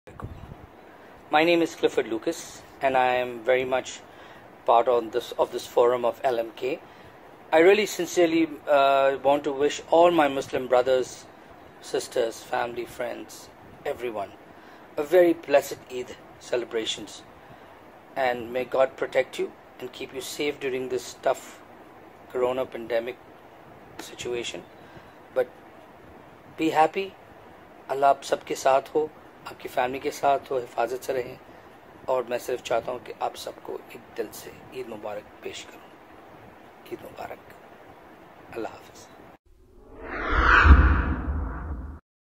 Clifford Lucas - LMK Eid Mubarak
Clifford Lucas - vocalist and keyboardist of The InTime band shares a special Eid Mubarak message to all.